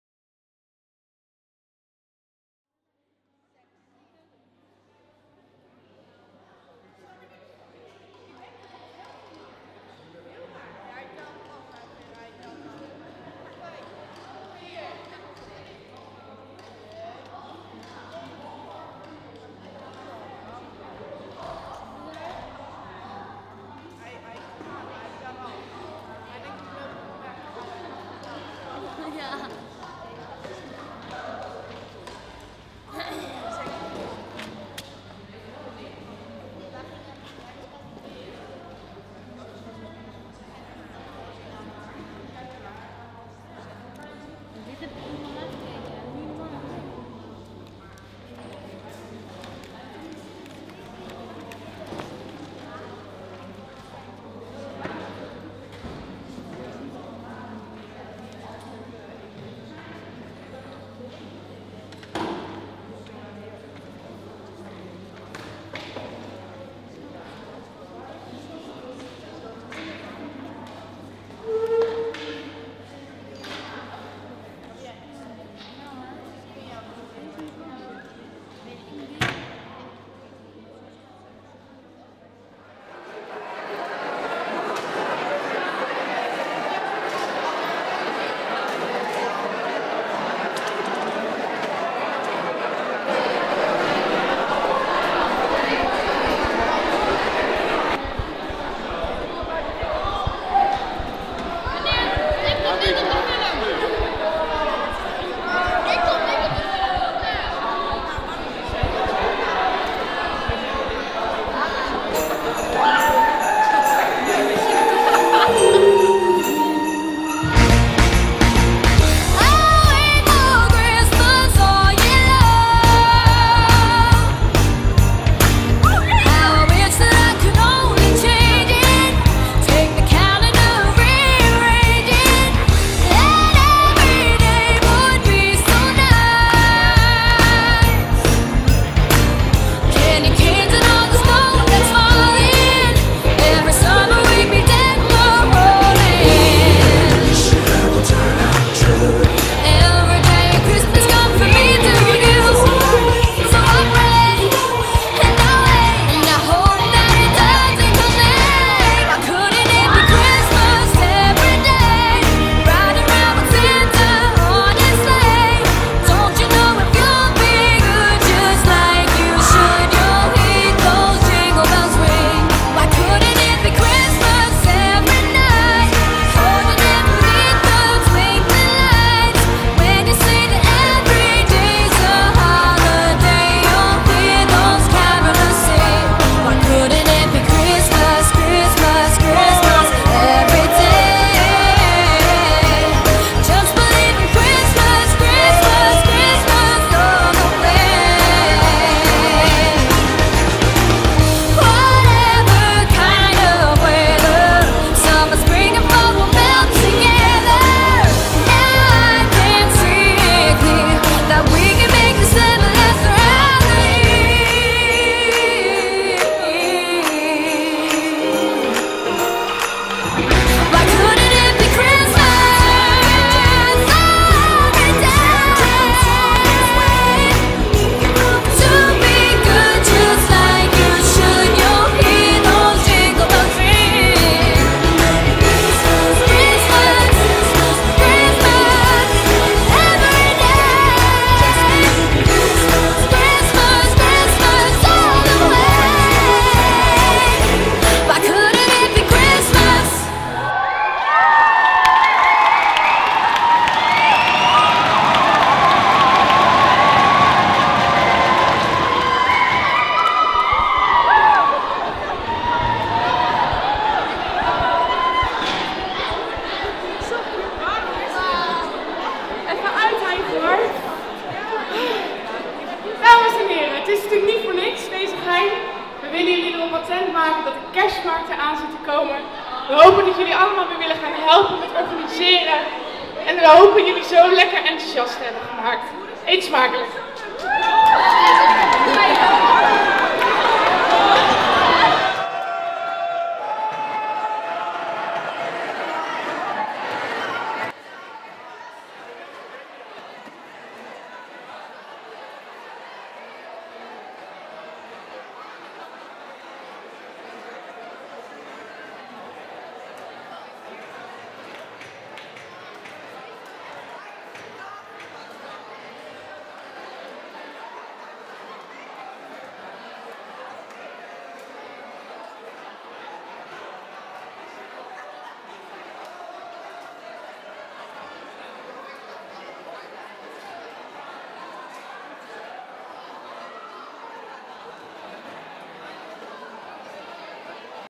Flashmob Lentiz Dalton MAVO
Fashmob op de Lentiz Dalton MAVO. Dit is aankondiging van de kerstmarkt. Komen jullie ook op donderdag 13 december?